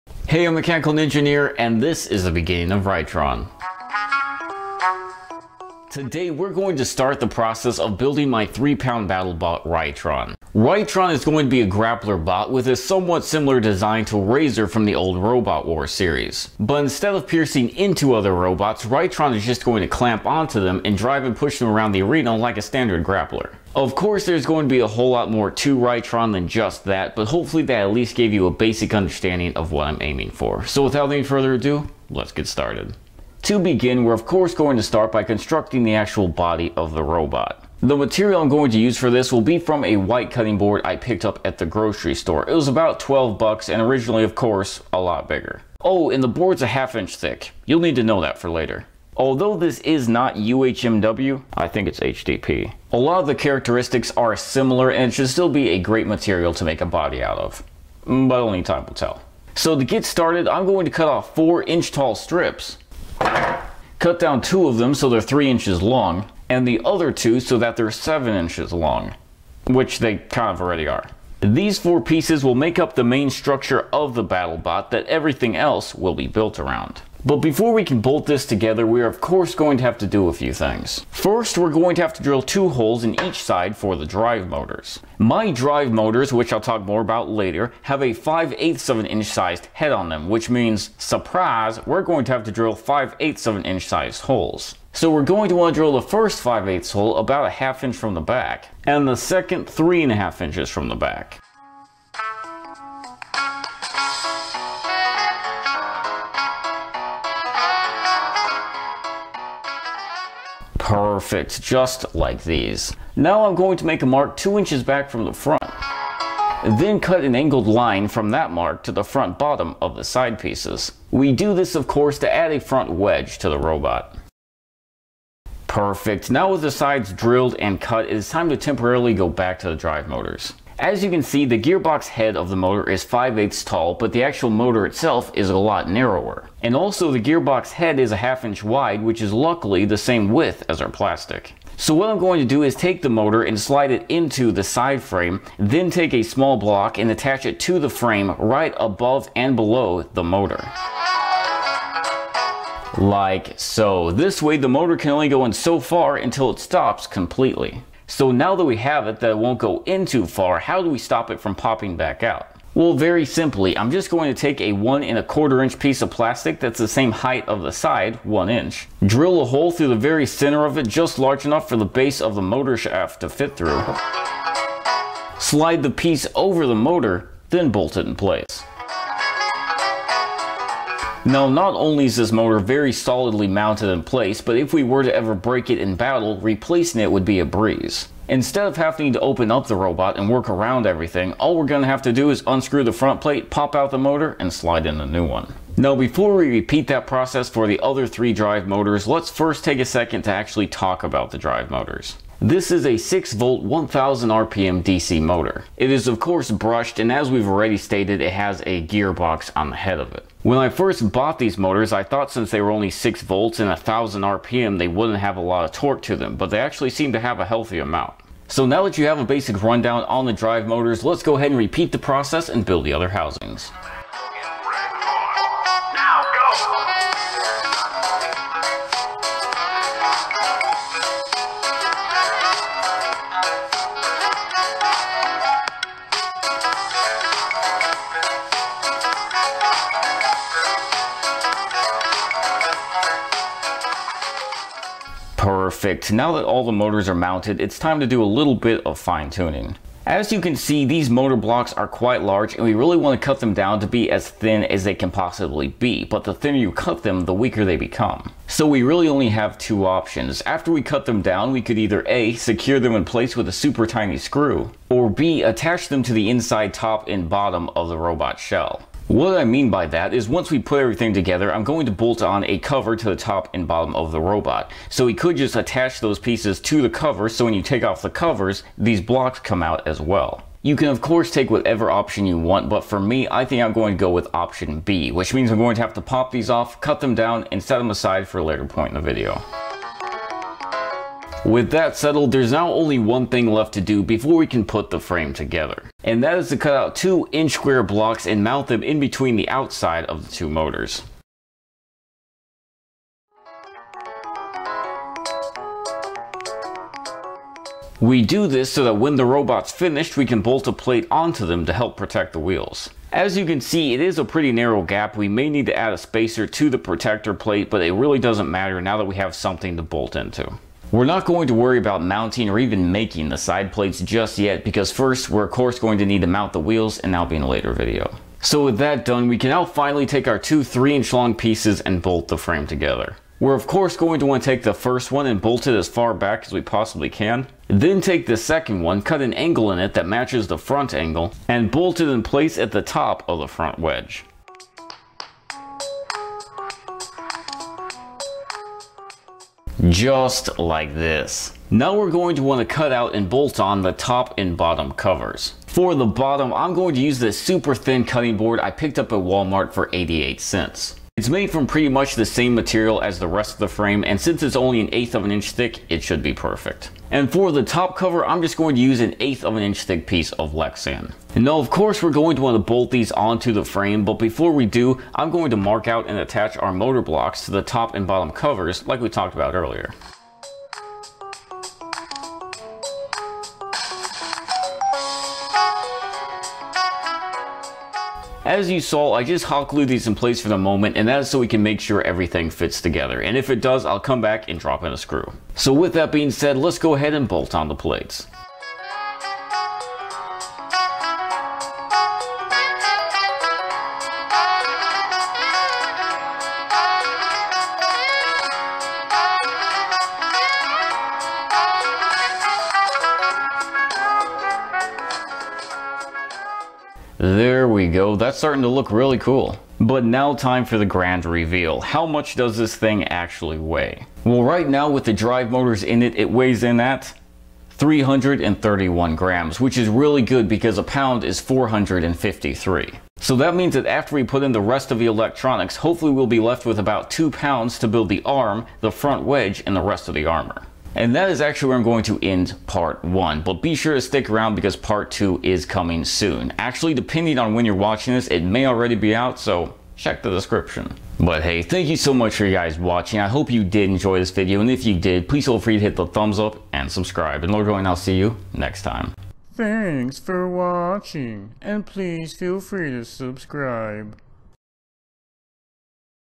How To Build A 3lb Battlebot!!! // Ritron Part 1/4
Today we will start the process of building my 3lb battlebot "Ritron"! Ritron is going to be a grappler robot with a focus on drive torque! :D

Drive Motors